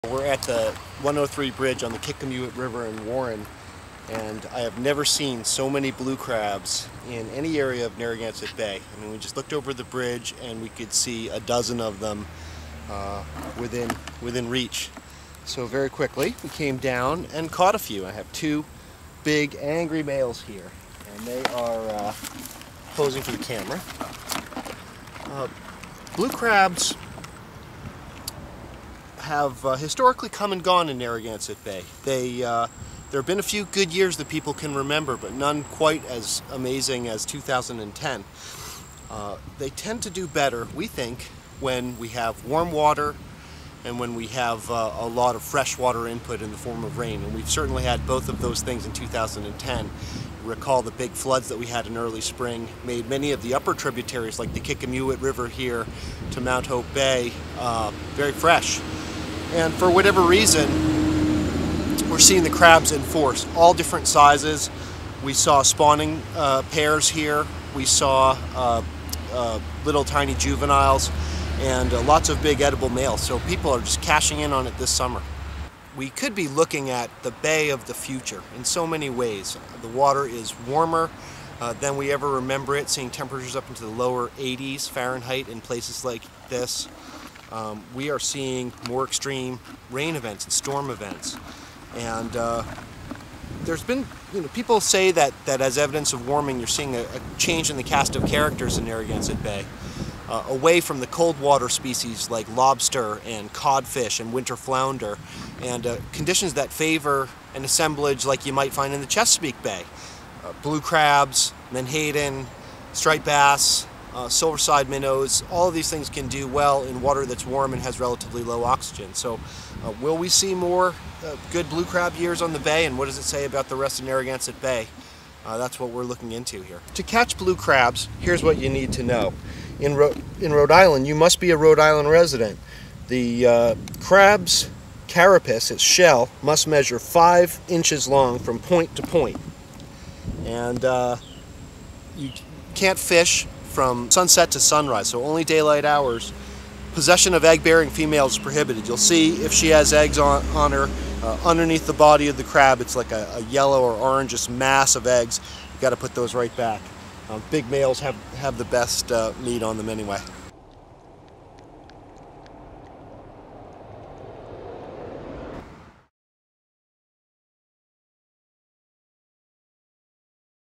PROTECT: Blue Crabs Everywhere!
Blue crabs are out in record numbers. Save The Bay's John Torgan goes fishing for answers why.